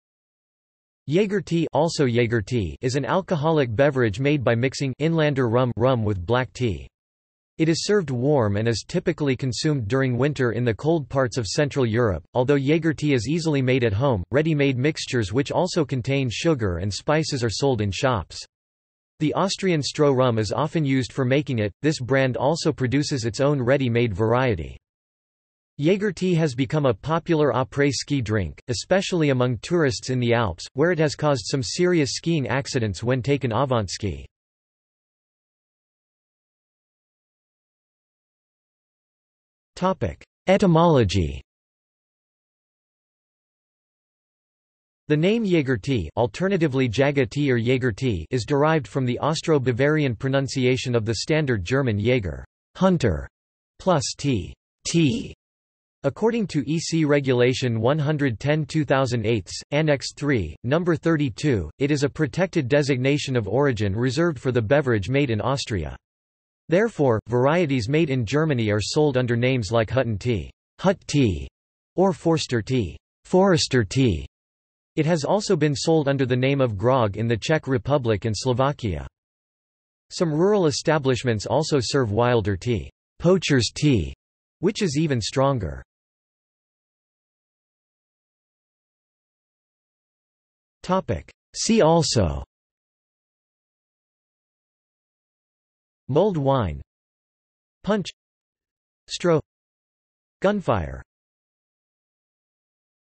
Jagertee | Wikipedia audio article
This is an audio version of the Wikipedia Article:
Jagertee

00:00:36 undefined
00:01:27 undefined

- Socrates

SUMMARY
Jagertee (also Jägertee) is an alcoholic beverage made by mixing “Inlaender-Rum” rum with black tea. It is served warm and is typically consumed during winter in the cold parts of Central Europe.Although Jagertee is easily made at home, ready-made mixtures which also contain sugar and spices are sold in shops. The Austrian Stroh rum is often used for making it; this brand also produces its own ready-made variety.
Jagertee has become a popular après-ski drink, especially among tourists in the Alps, where it has caused some serious skiing accidents when taken avant-ski.